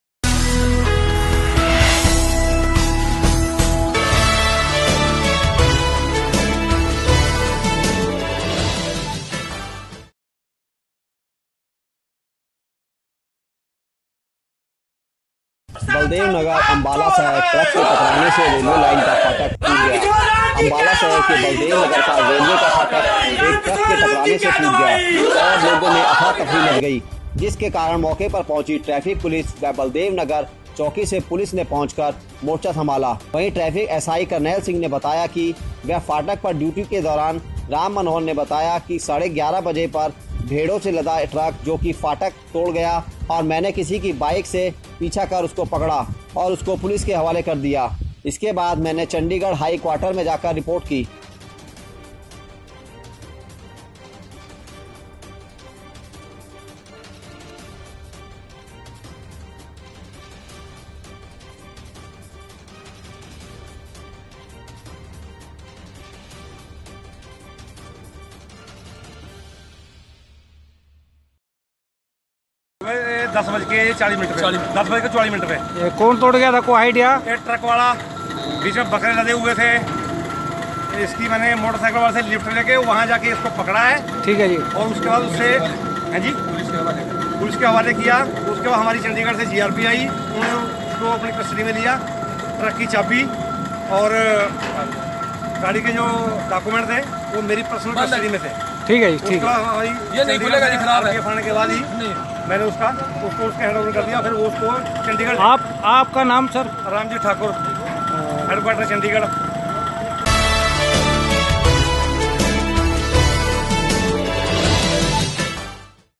बलदेव नगर अंबाला शहर ट्रक के टकराने से रेलवे लाइन का फाटक टूट-Today24
अंबाला शहर बलदेव नगर का रेलवे फाटक एक ट्रक के टकराने से टूट गया और लोगों में अफरा-तफरी मच गई जिसके कारण मौके पर पहुंची ट्रैफिक पुलिस वह बलदेव नगर चौकी से पुलिस ने पहुंचकर के मोर्चा संभाला ट्रैफिक SI करनैल सिंह ने बताया बताया कि वह फाटक पर ड्यूटी के दौरान राममेहर ने बताया सुबह 11:30 पर भेड़ों से लदा ट्रक जो कि फाटक तोड़ गया और मैंने किसी की बाइक पकड़कर पीछा करके मौके पर उस ट्रक को पकड़ा वह पुलिस के हवाले किया और अपने चंडीगढ़ हाई क्वार्टर में जाकर रिपोर्ट की
रिपोर्टर शिव कुमार ब्यूरो चीफ के साथ कमल भसीन

T.V. News

Application.

views!!!

News."